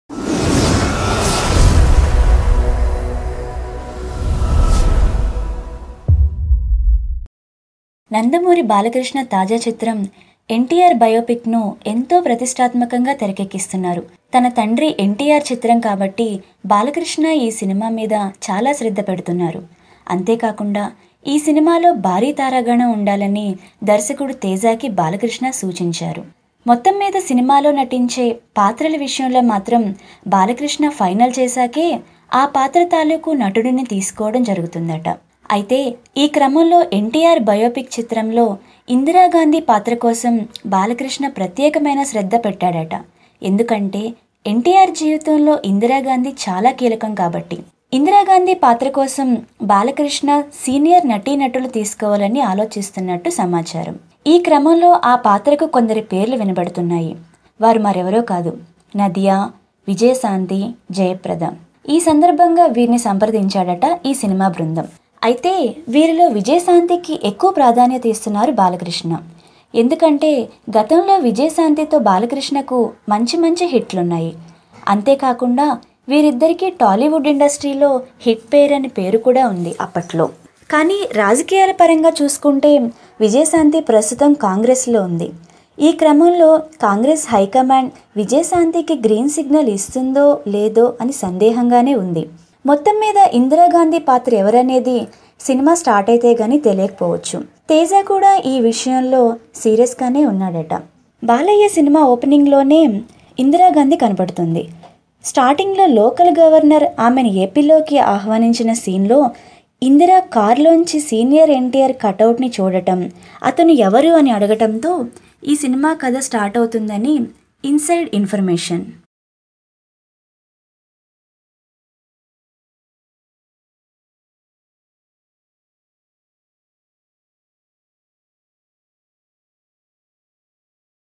ఇందిరా గాంధీ కోసం బాలయ్య సీరియస్ గా వెతుకుతున్నాడు! | Who Will Play Indira Gandhi Role In NTR Biopic
Who Will Play Indira Gandhi Role In NTR Biopic. Telugu Stars .Senior hero Balakrishna's upcoming movie is the most-prestigious biopic of legendary actor Nandamuri Taraka Ramarao. As the shooting still take some time to go on sets, Balayya has kept his entire focus on the film. He is taking some vital decisions by collaborating with the direction department. It seems like Balayya's will be the final call about the cast in the film. As per the talk, some artists are already finalized for this film. As Indira Gandhi played an important role in NTR's life, the team approached some senior actresses to play the role. If the buzz is to be believed, the veteran actresses Vijaya Shanthi who is like a c/o address for female-centric movies is playing Indira Gandhi role in NTR's biopic. The rumors claim that Balayya is also interested to cast Vijaya Shanthi . According to latest buzz from T-Town, actress Nadiya has been approached to play the role of India’s first and only woman Prime Minister Indira Gandhi.The makers of Balakrishna’s next, NTR biopic are leaving no stone unturned to make the film a realistic one by casting top actors.when late CM NT Rama Rao faces the crucial period during his initial political days, the role of Indira Gandhi was important. Thus they approached Nidiya who is a performer as well as a stylish actor to suit the late PM.ఇందిరా గాంధీ కోసం బాలయ్య సీరియస్ గా వెతుకుతున్నాడు